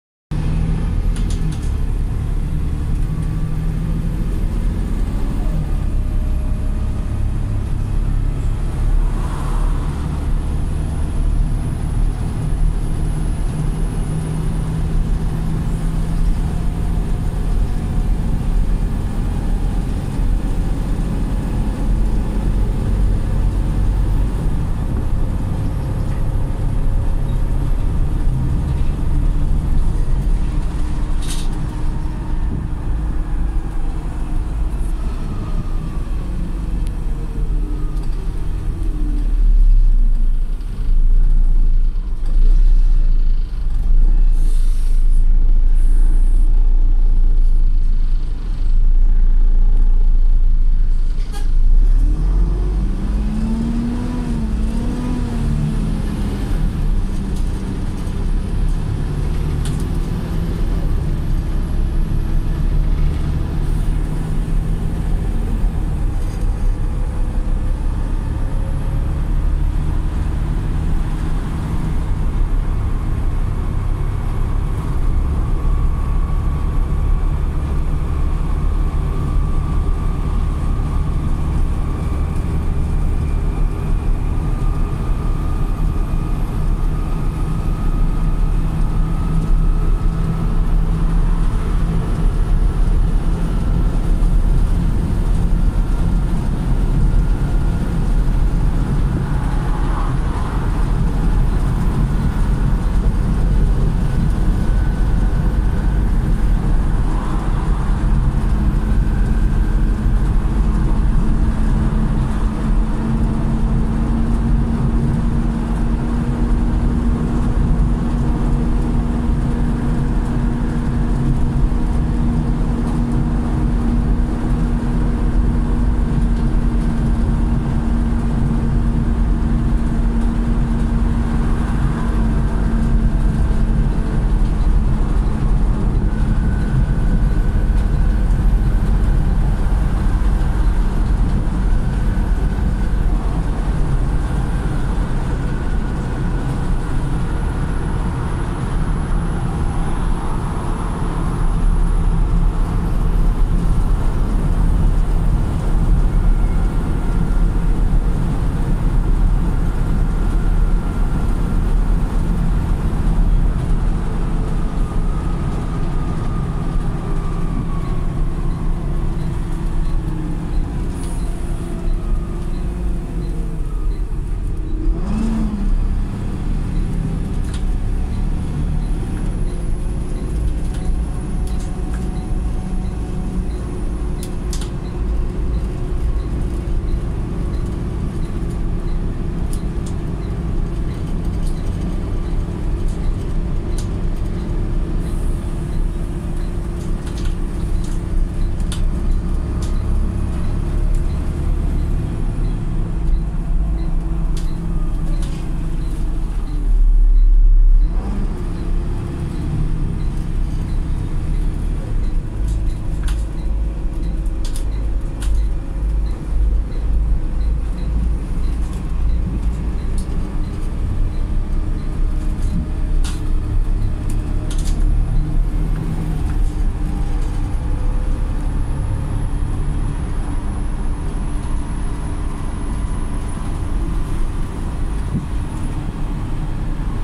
Malahide Coaches - ALX400 Volvo B7TL 7.3L (Ex Dublin Bus AV329) - LUAS Replacement Shuttle Bus
In this video I recorded my journey on a former Dublin Bus AV class on the LUAS Green Line Replacement Shuttle Bus Service from Brides Glen to Carrickmines this Easter Sunday.